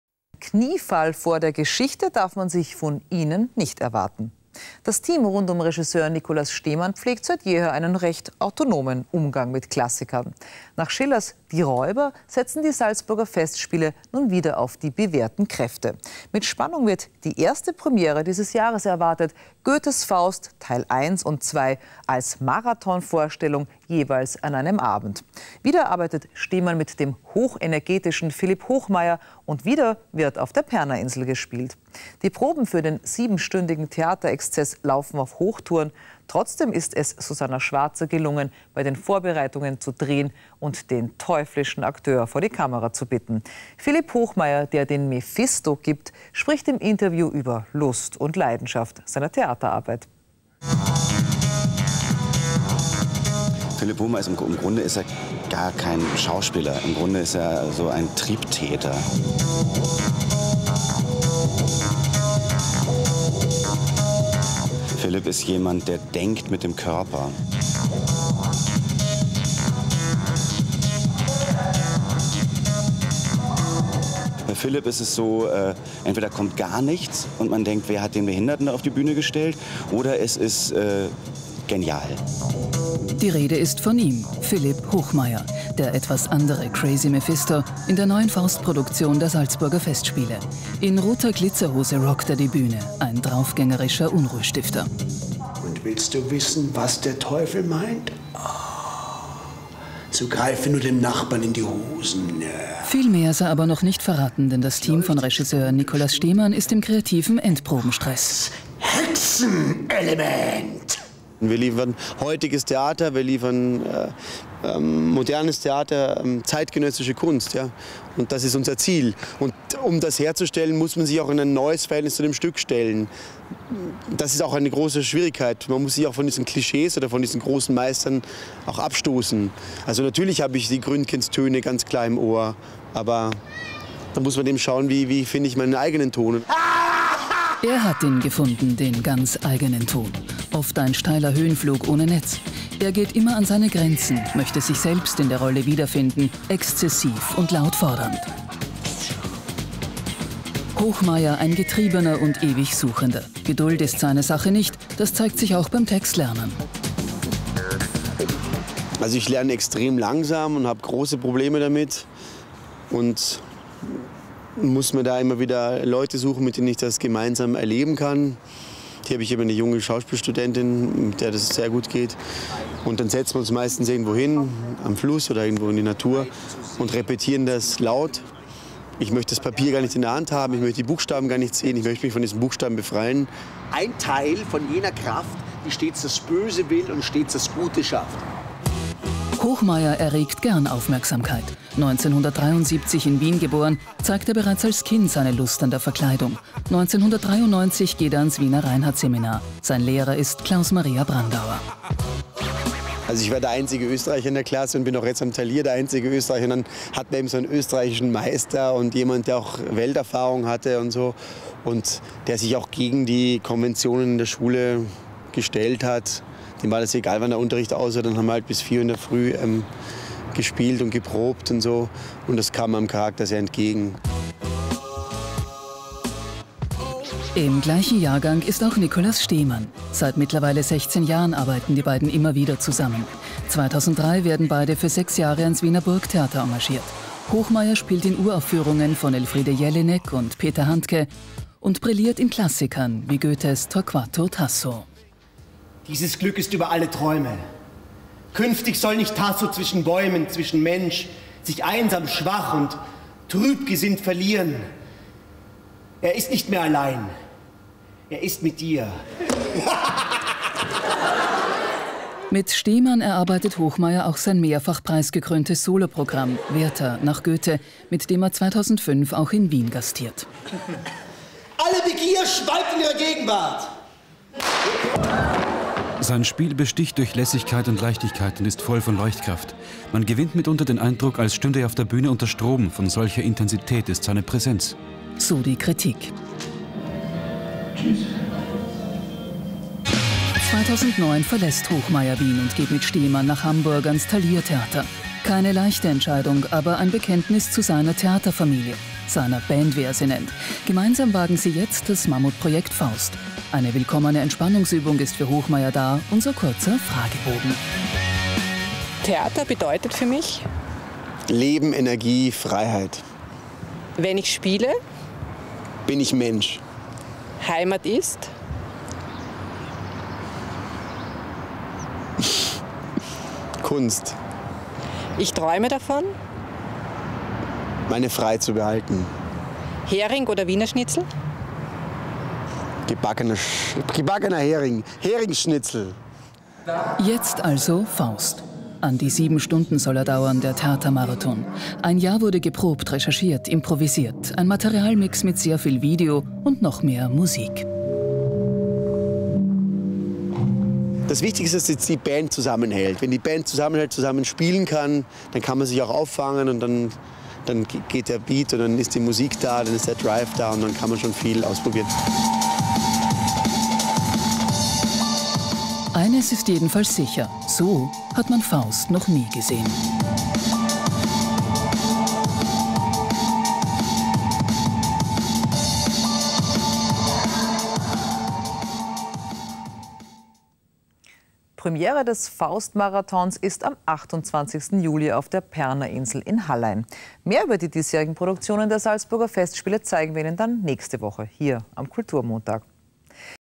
Philipp Hochmair spielt Mephisto bei den Salzburger Festspielen 2011
Portrait des Schauspielers Philipp Hochmair anlässlich der Premiere von Goethes Faust bei den Salzburger Festspielen 2011